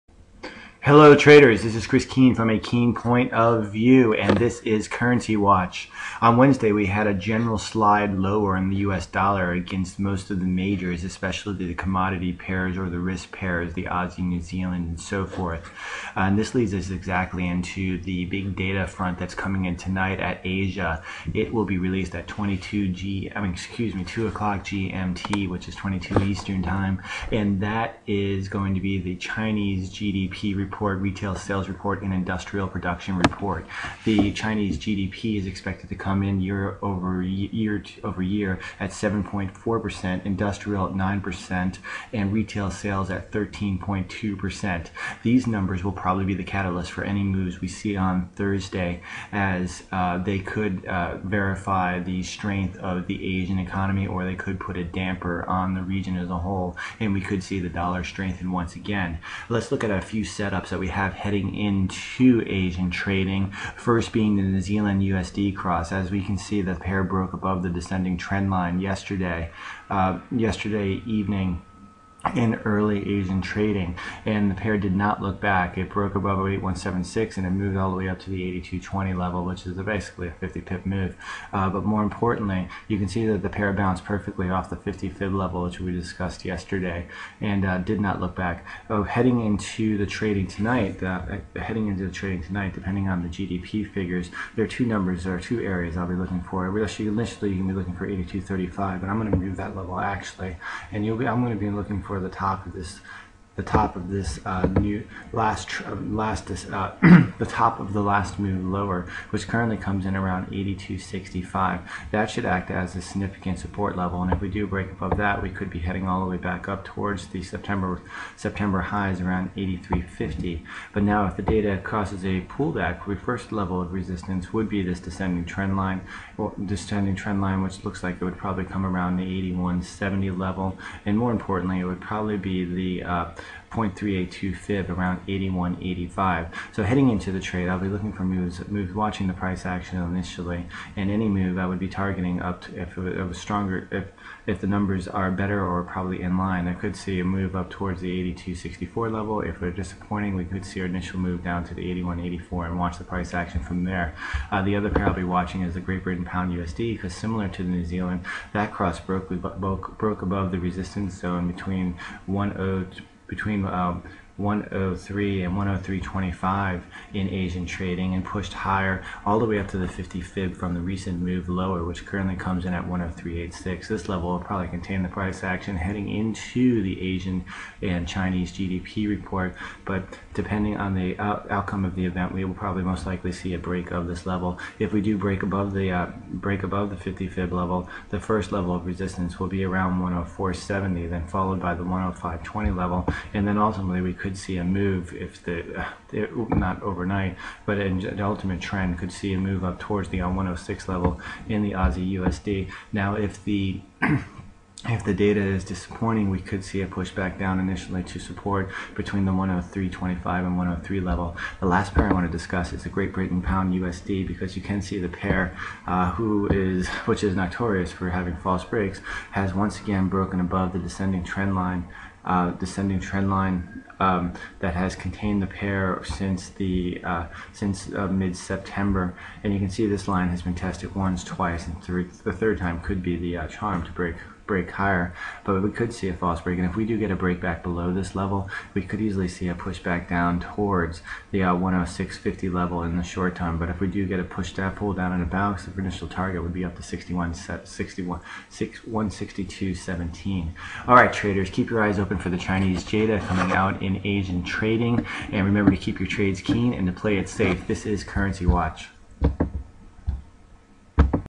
Currency Watch...Thursday Trading Plan...China Data on the way
A look at the NZDUSD AUDUSD and GBPUSD heading into Thursdays Trading